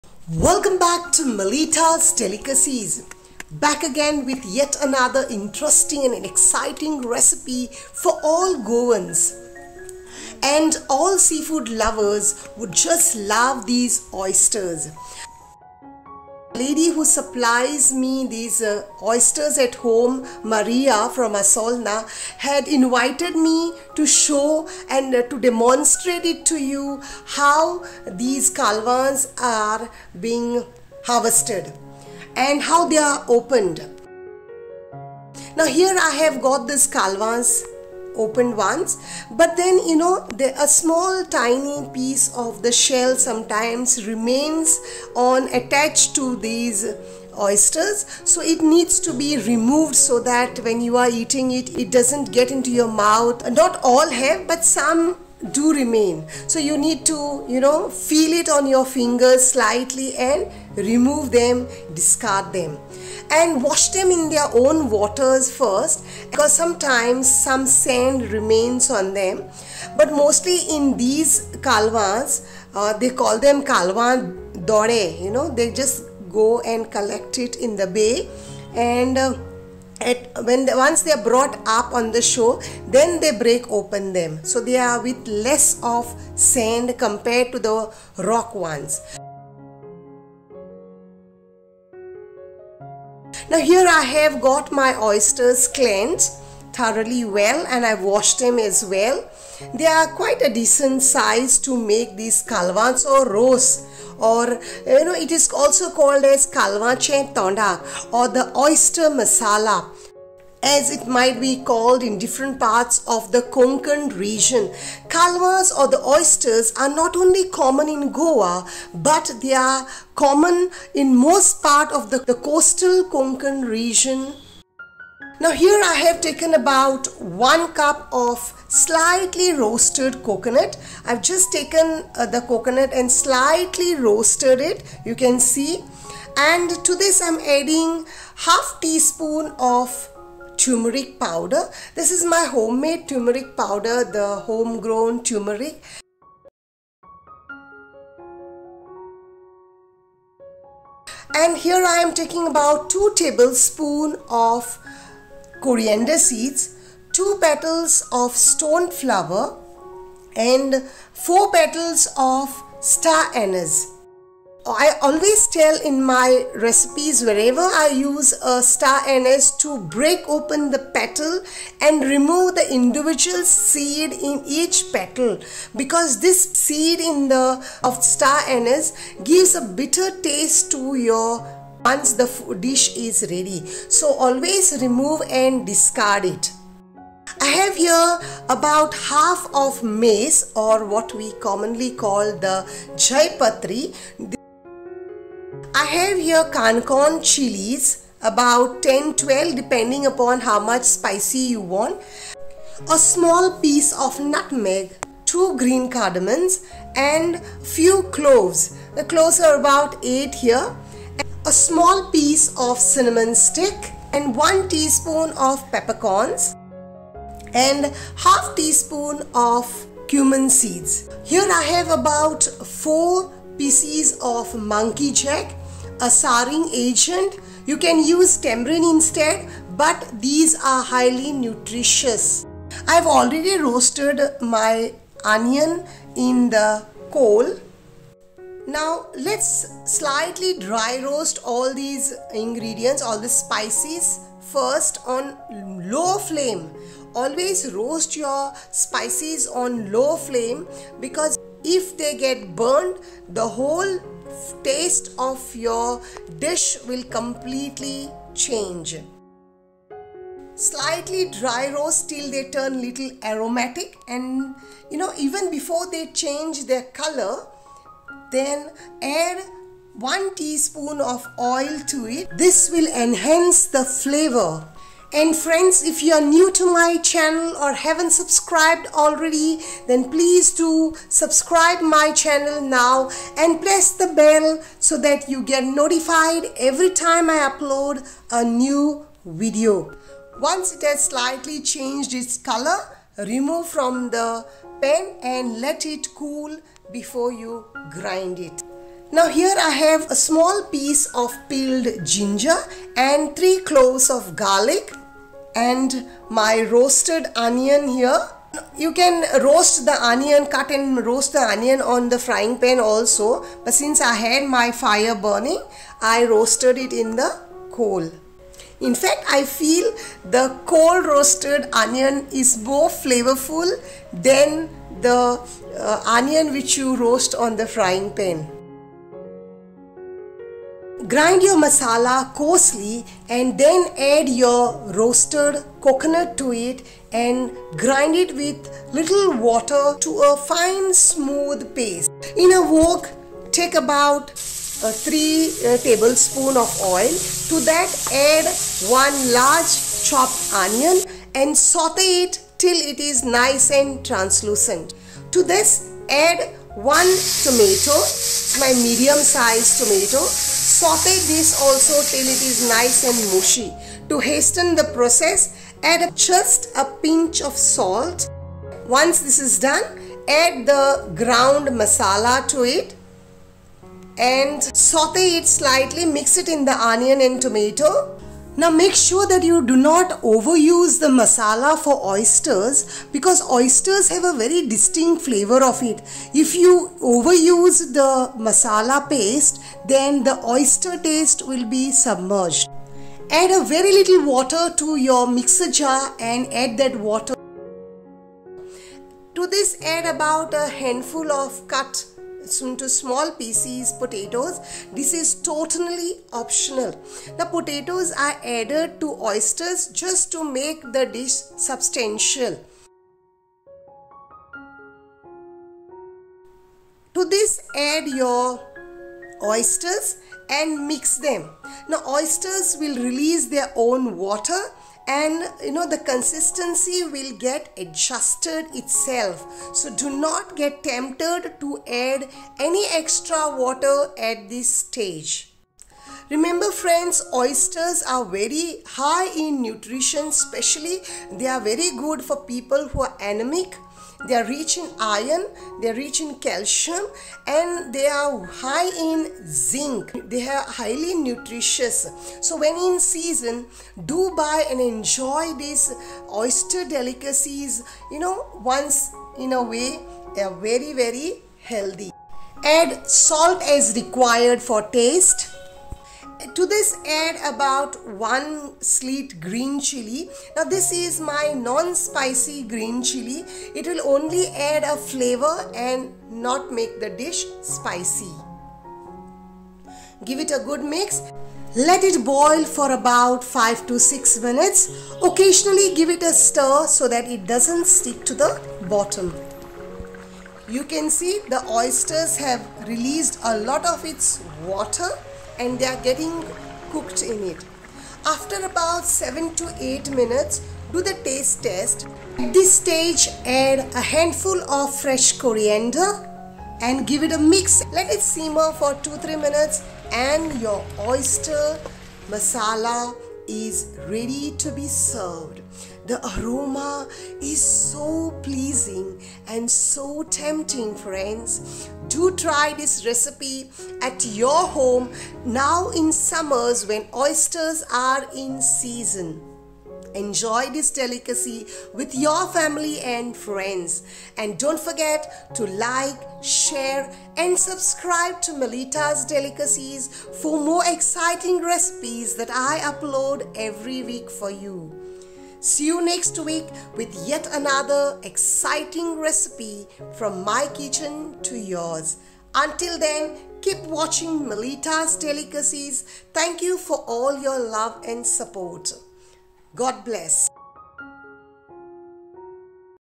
Oyster Masala#Oyesters|Kalwache Tonak|Goan Style Oyesters Masala|Oyster Currry#GoanSeaFood
Oyesters are called Kalva Or Kalwa in Goa and Konkan region and they just taste amazing in any recipe. Let's learn step by step quick and easy method to prepare Oyester Masala Goan style prepared by using fresh coconut for gravy with roasted aromatic spices.

Share with your family and friends too.

Melita's Delicacies